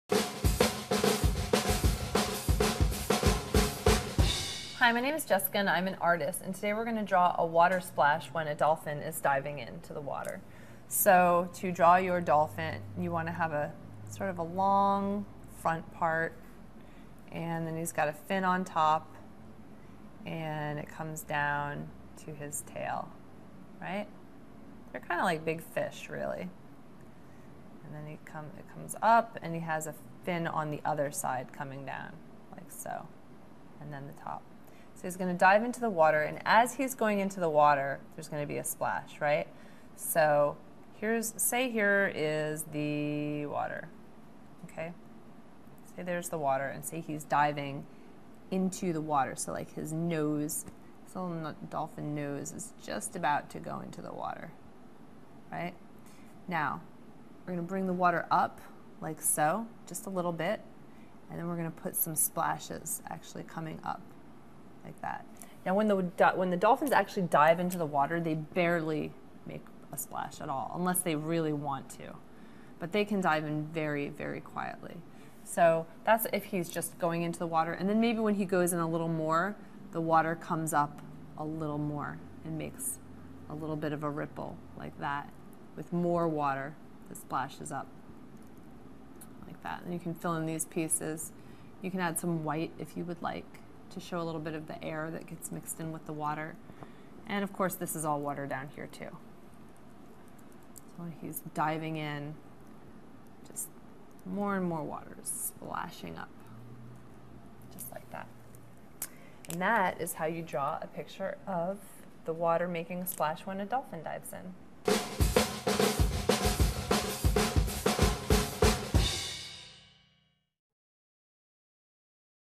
How to Draw a Water Splash When a Dolphin Is Diving In
How to Draw a Water Splash When a Dolphin Is Diving In. Part of the series: How to Draw Nature. Water splashes after a diving dolphin in a very specific way due to the animal's shape. Learn how to draw a water splash when a dolphin is diving in with help from an artist in this free video clip.